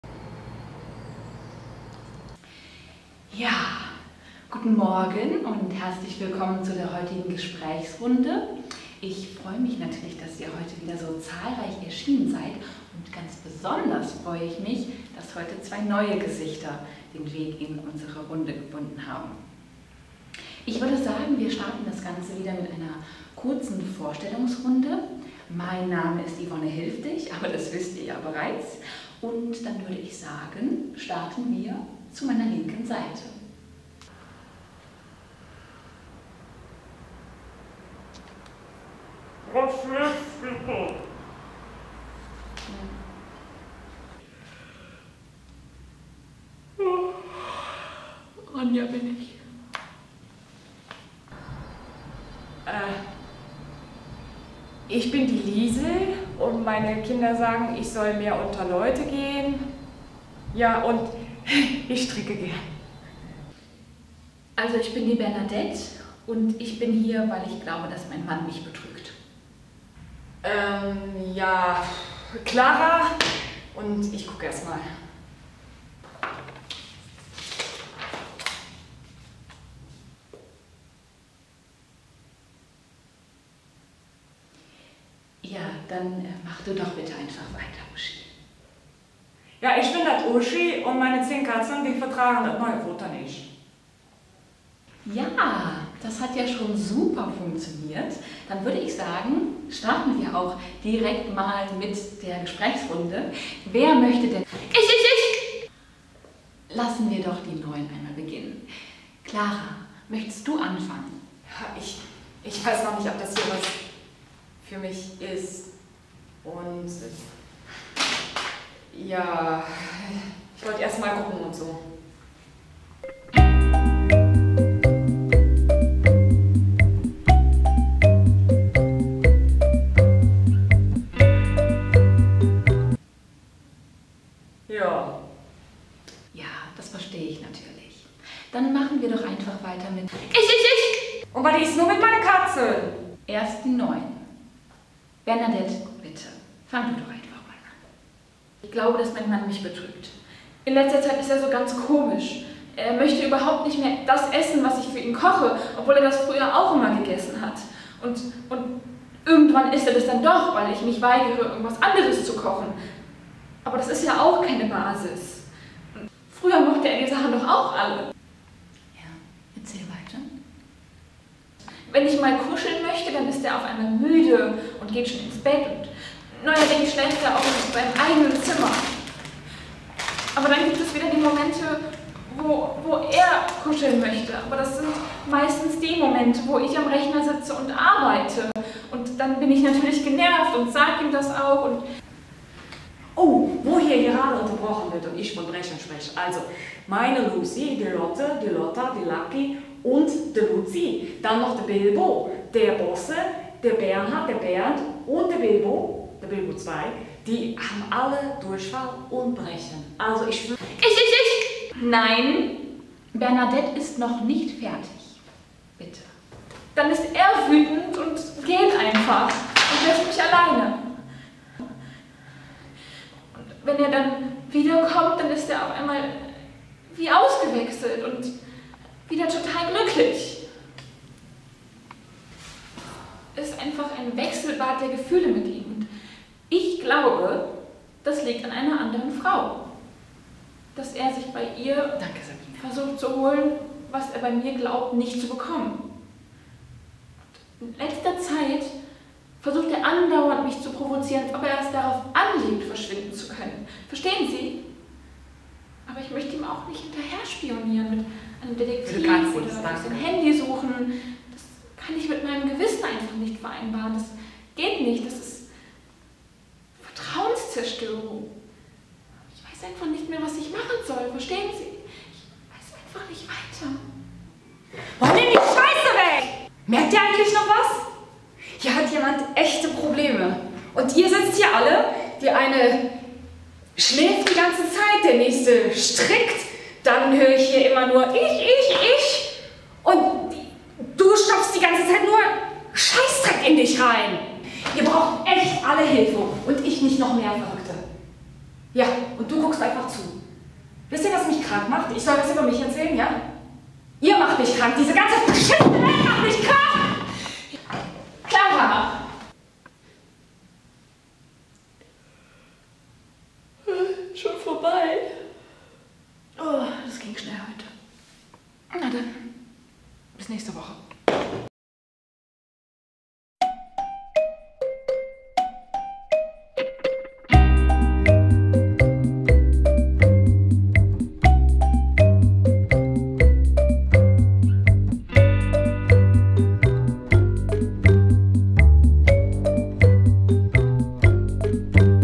Gruppentherapie bei Ivonne Hilfdich - Folge 1
In der heutigen Therapiesitzung begrüßt Ivonne Hilfdich die beiden neuen Teilnehmer Clara und Bernadette in ihrer Runde.
Wie werden sie auf die immer hungrige Sabine, die müde Ronja, die strickende Liesel, die abgelenkte Hyper und die Katzenmami Uschi reagieren? Schaut das Video und findet es heraus!

Ihr wollt mehr? Dann liked und kommentiert dieses Video und wer weiß, vielleicht wird es bald eine Fortsetzung geben ;)